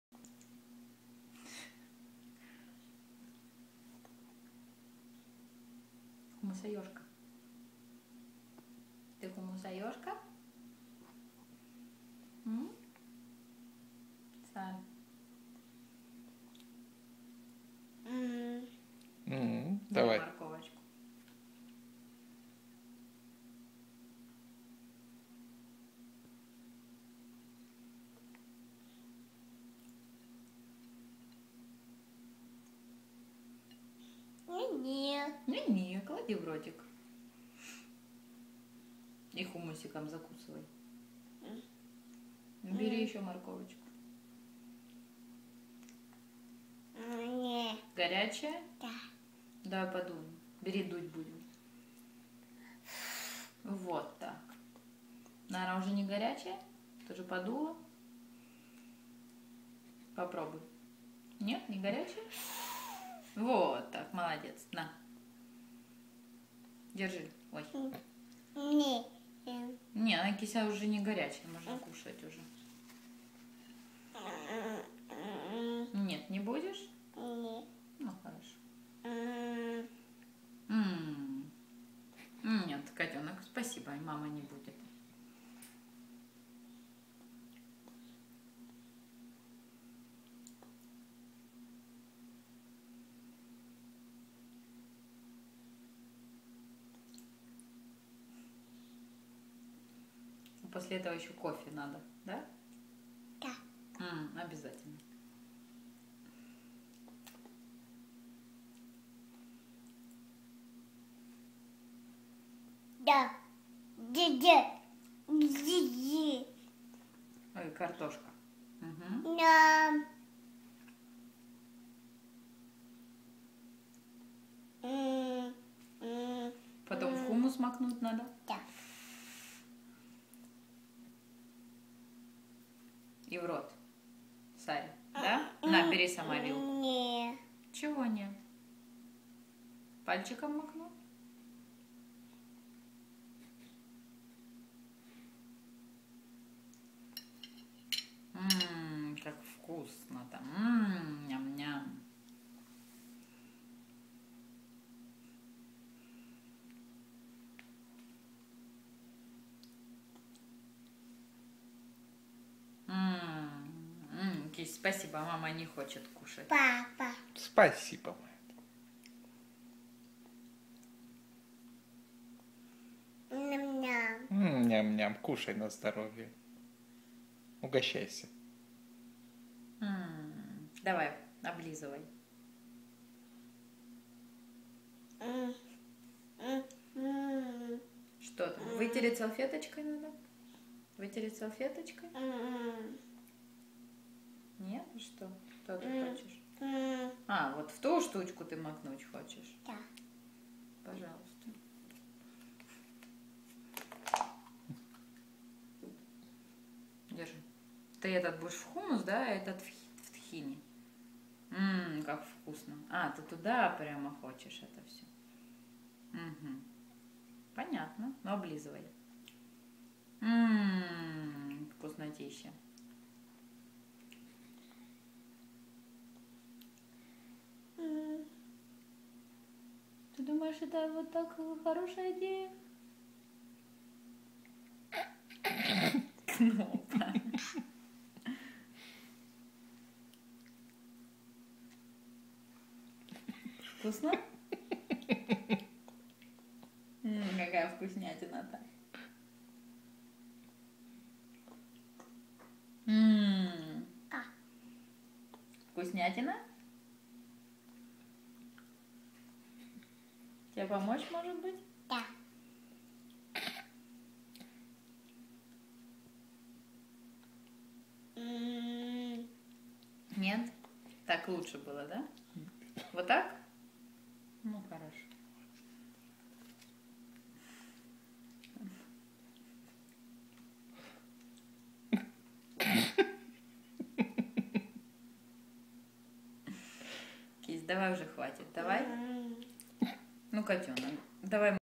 Сара с хумусом
Сара кушает хумус и запивает тхини